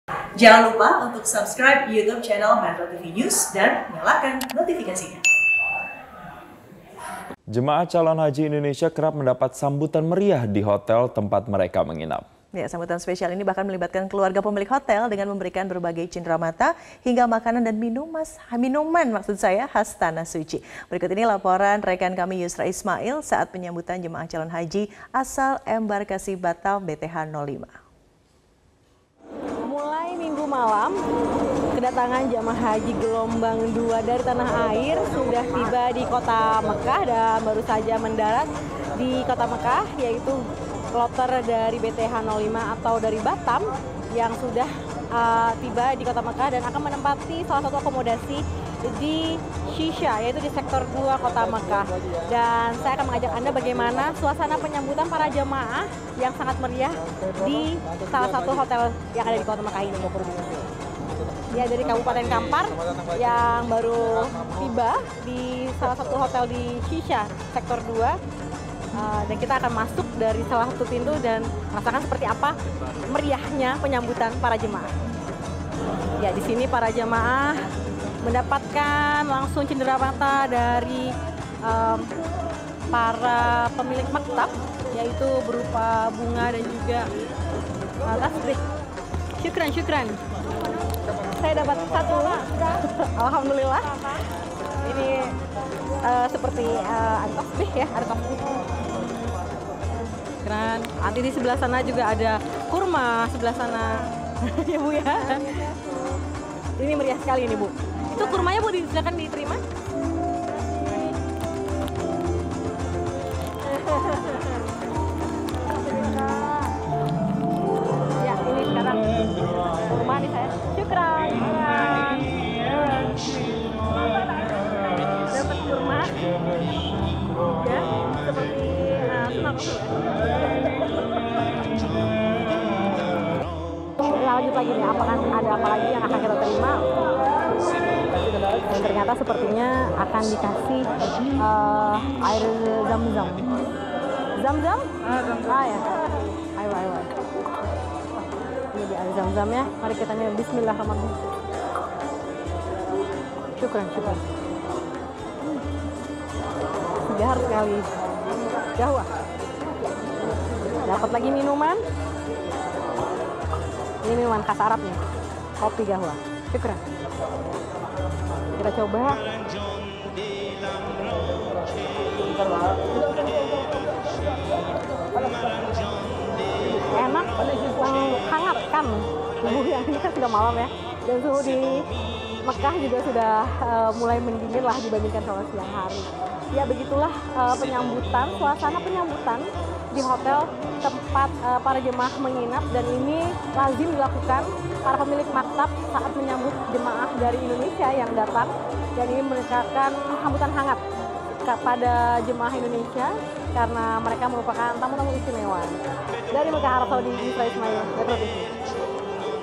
Tiba di Kota Makkah, Jemaah Calon Haji Indonesia Kloter 5 Embarkasi Batam Disambut Meriah
Jemaah calon haji Indonesia kloter 5 Embabarkasi Batam kerap mendapat sambutan meriah di salah satu hotel di Kota Makkah, tempat mereka menginap. Sambutan spesial ini bahkan melibatkan keluarga pemilik hotel dan memberikan berbagai cinderamata hingga makanan dan minuman khas tanah suci. Kira-kira bagaimana kemeriahannya?